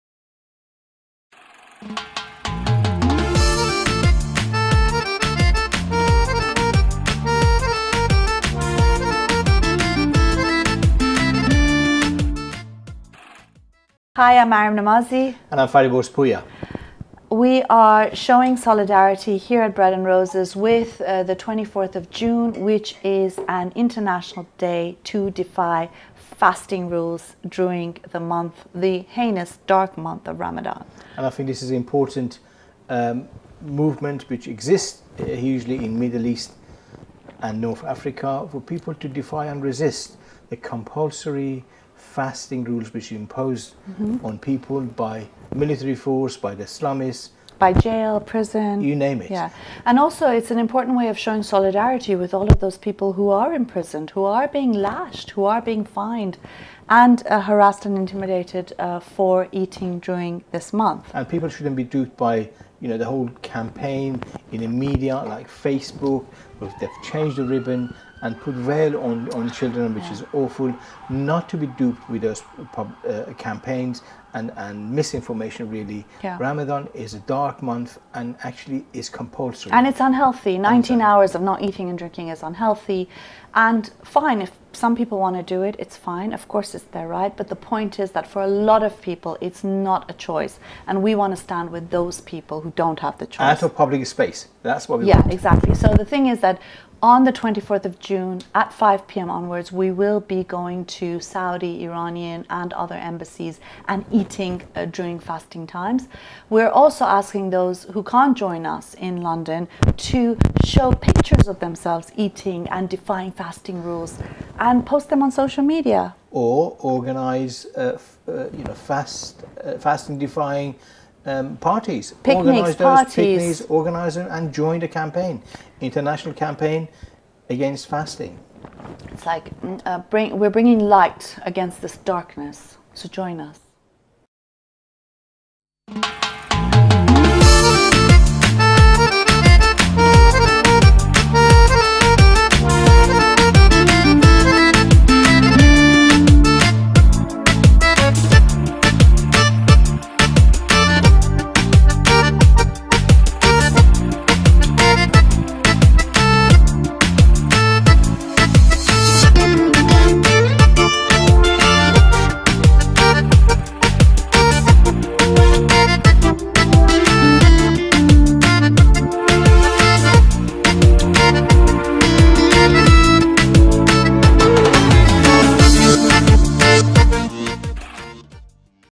24 June 2016: Eat-In for Ramadan
On 24 June, from 12:00, the Council of Ex-Muslims of Britain will be organising an "eat-in" at the Saudi, Iranian, Bangladeshi, Pakistani... embassies in London in solidarity with those defying fasting rules during Ramadan. This is hugely important given that there are many people across the globe who are arrested, beaten and fined for eating during the month; many others are pressured into fasting, including in Europe. Join us at the 'eat-in' if you can.

Alternately, you can upload photos of yourself eating during fasting times or holding signs with messages of solidarity using hashtag: IWillNotFast لن_اصوم روزه خوارى Ramadan until the end of Ramadan. Happy fast-defiance!

CREDITS
This video is Creative Commons, which means anyone can repost it.